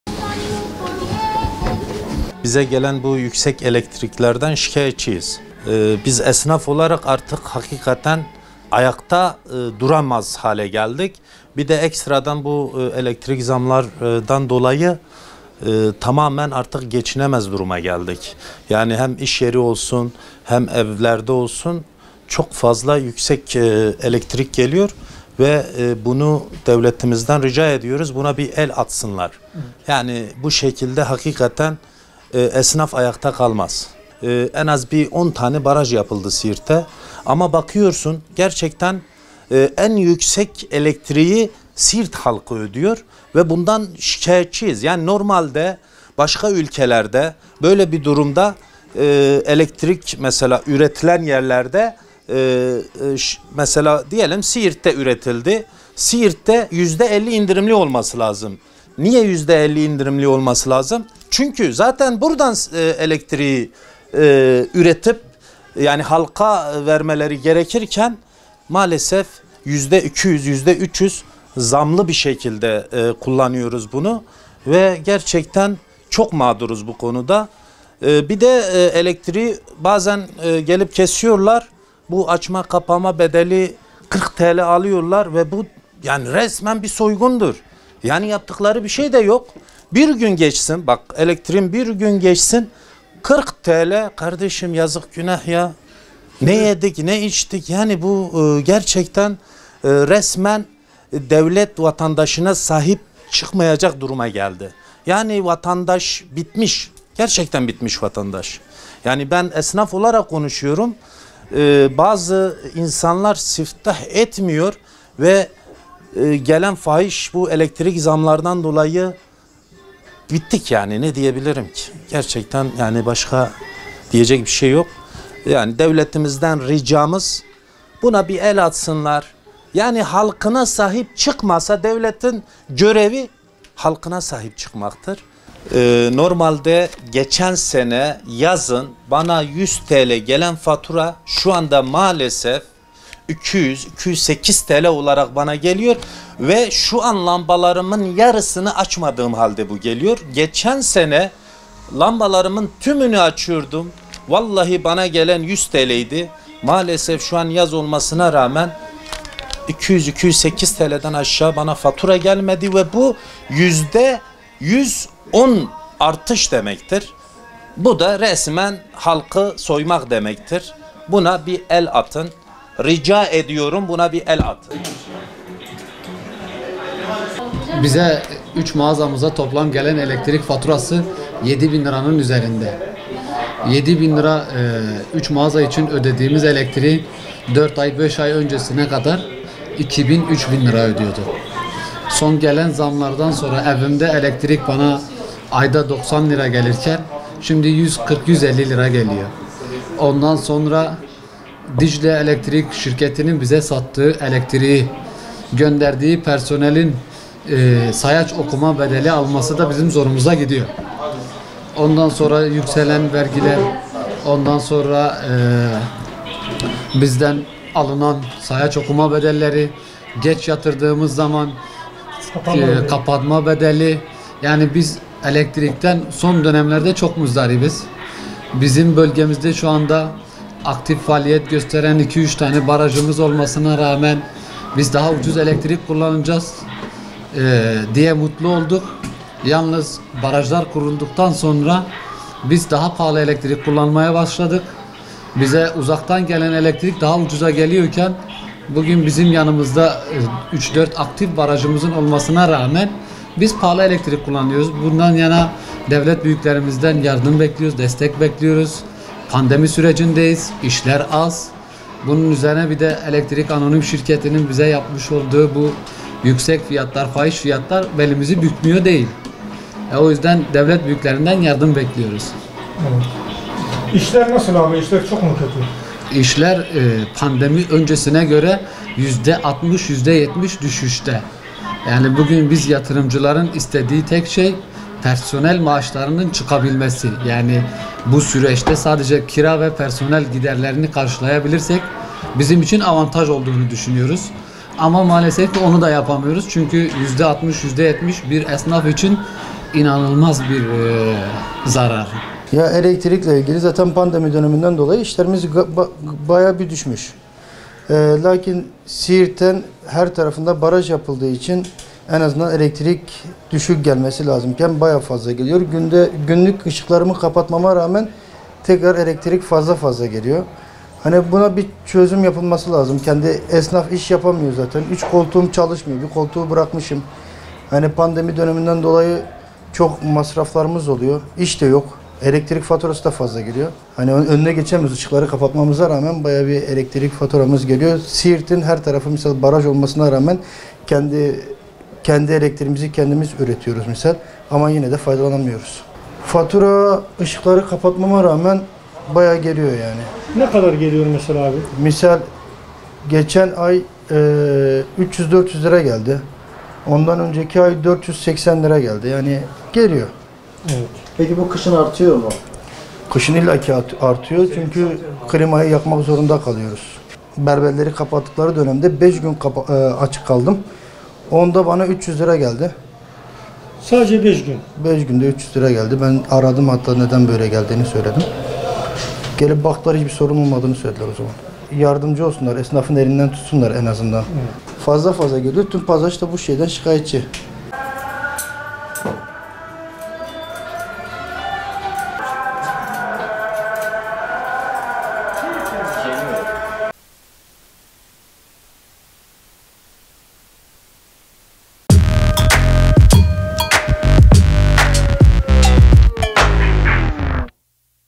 Esnaf Elektrik Faturasına İsyan Etti
Son zamanlarda gelen yüksek elektrik faturaları esnafı mağdur etmeye devam ediyor.